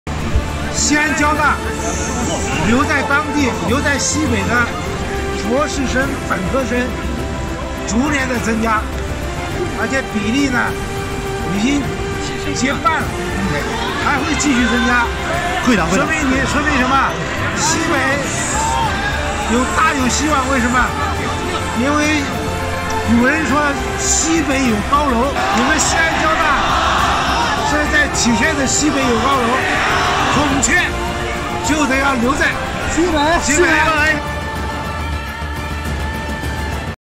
今天总理来交大！遇见·李克强【字幕版】
2019年10月15日上午，中共中央政治局常委，国务院总理李克强到西安交通大学调研。

教育部党组书记、部长陈宝生， 陕西省委书记胡和平、省长刘国中等陪同调研。路两边围满了同学，大家争相与李克强总理握手，用手机留下珍贵的照片和视频。

总理寄语，西北有高楼，上与浮云齐。愿为双鸿鹄，奋翅起高飞。勉励交大学子努力奋斗！